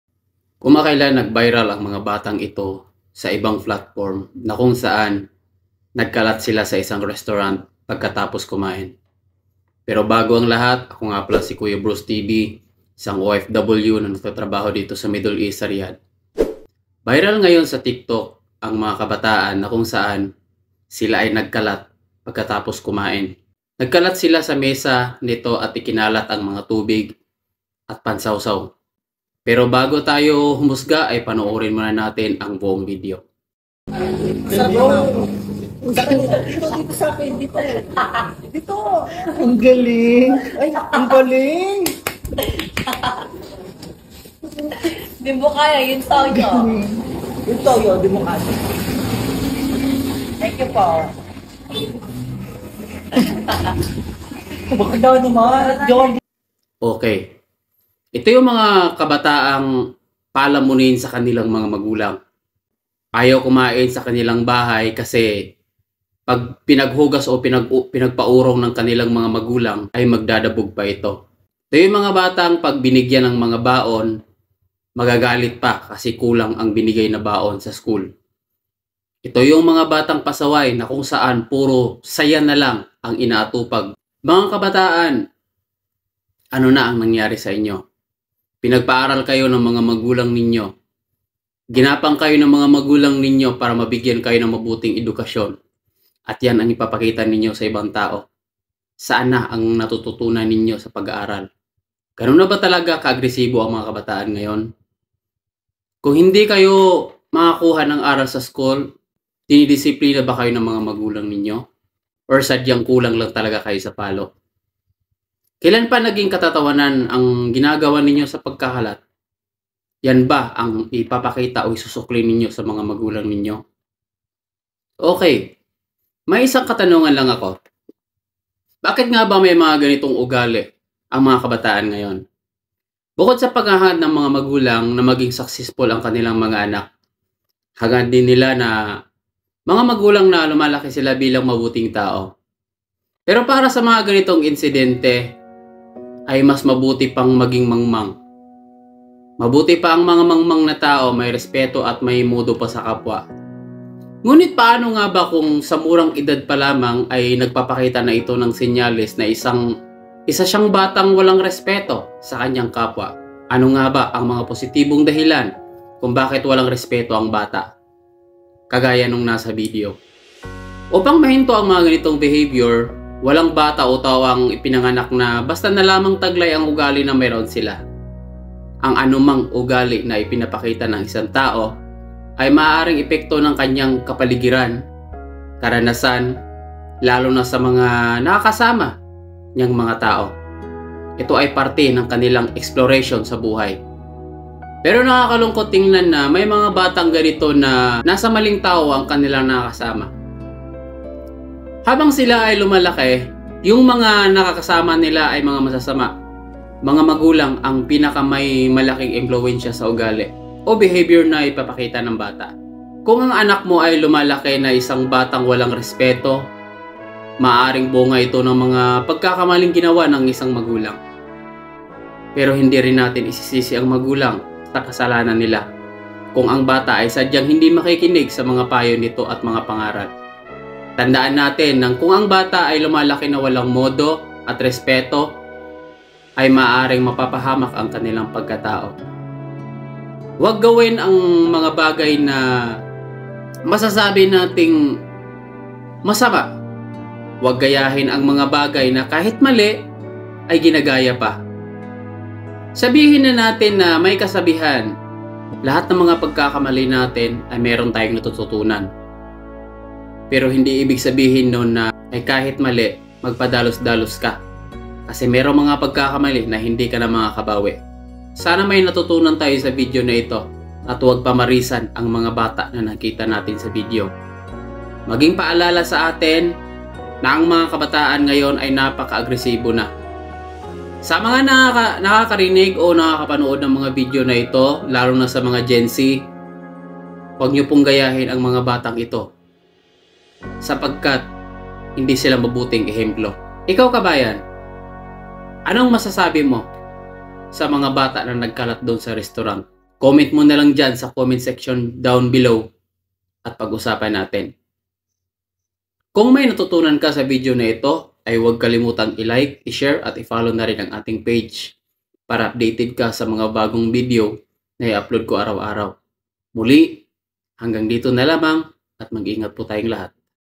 MGA BATANG PASAWAY NAGKALAT SA RESTORANT | -ikinalat ang toyo at tubig sa mesa pagkatapos kumain.
ccto